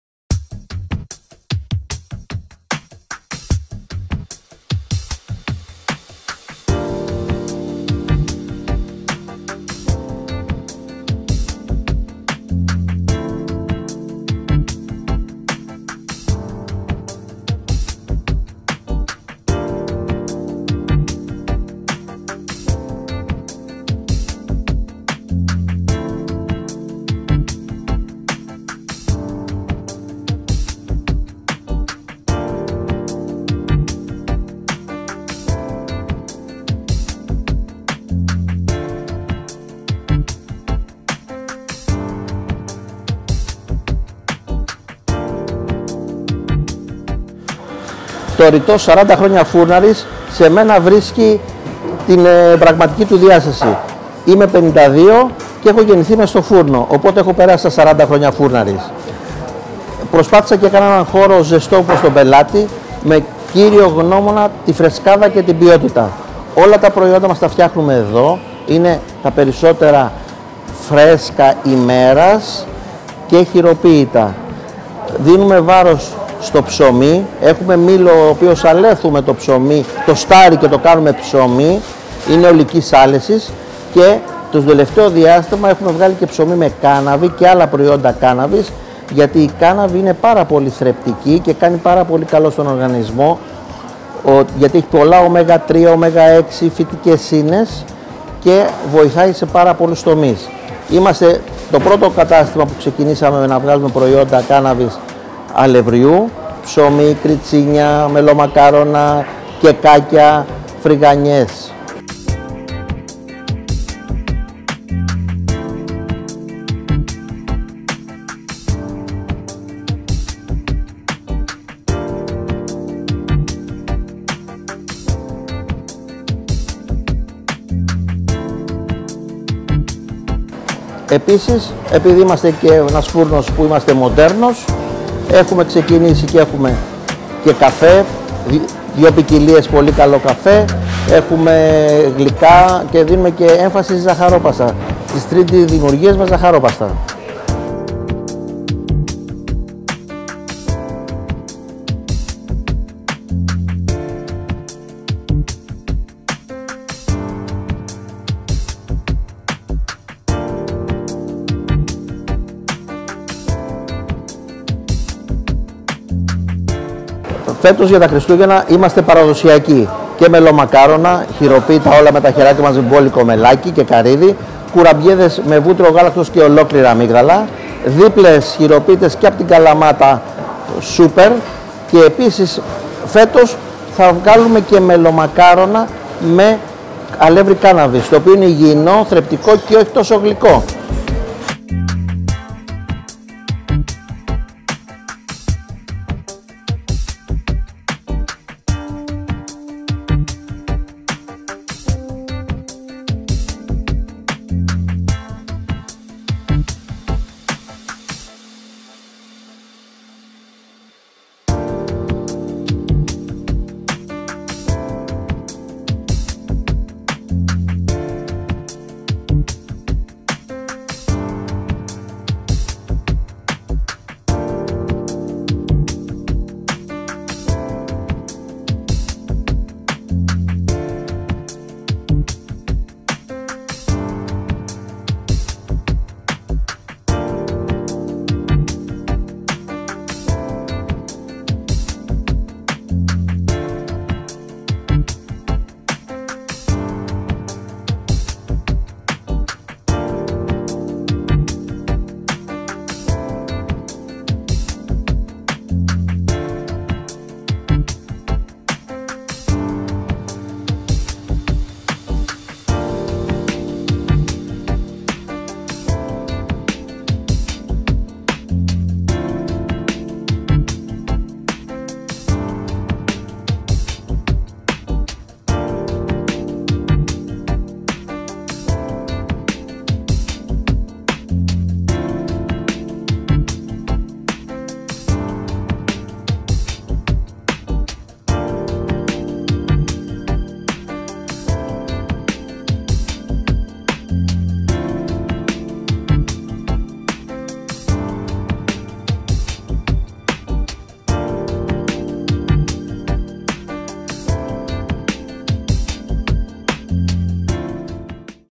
Βαζάκας / Σφραγίδα ποιότητας
Για δεκαετίες τώρα ο φούρνος 'Βαζάκας' βρίσκεται δίπλα στις ανάγκες των καταναλωτών του!
Μοναδικές γεύσεις, ψωμί, κριτσίνια, γλυκά και ότι άλλο τραβά η όρεξη σας!

Ο πρώτος φούρνος με Ψωμί από Κάνναβη στο Βαζάκα!
Δείτε το βίντεο και μάθετε όλες τις λεπτομέρειες!

Σοφοκλή Βενιζέλου 80
Λυκόβρυση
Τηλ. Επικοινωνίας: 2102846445